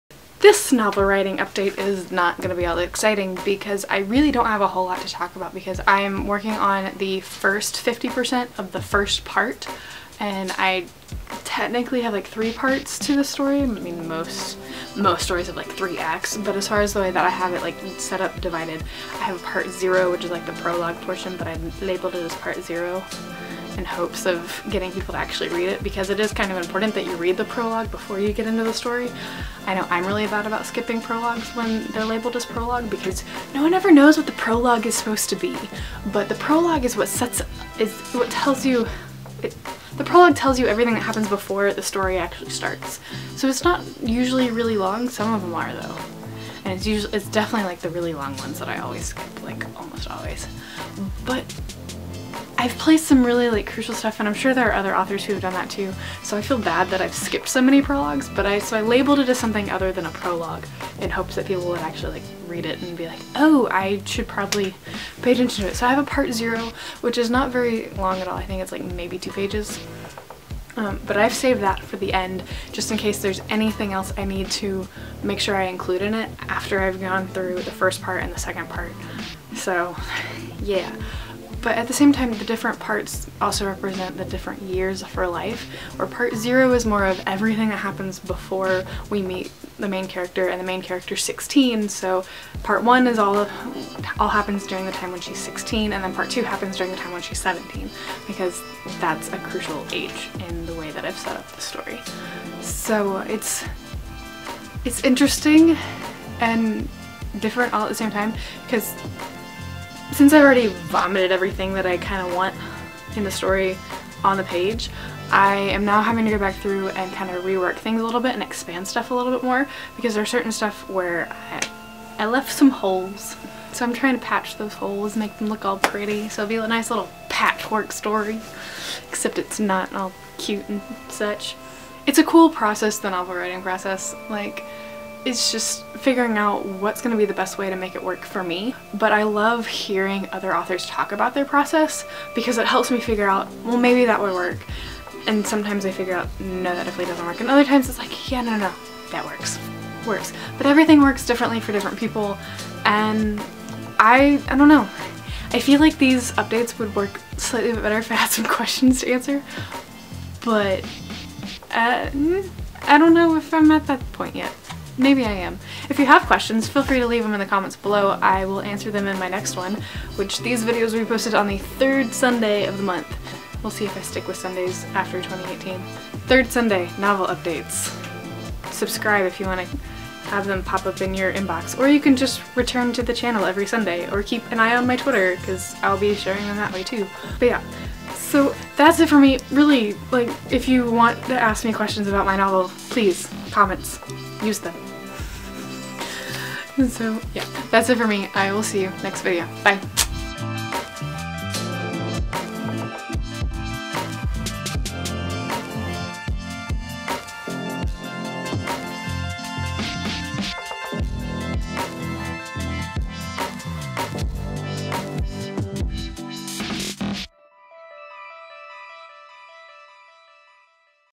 Breaking It Down | Novel Process Ep. 5
Breaking down the structure of how I've set up my novel.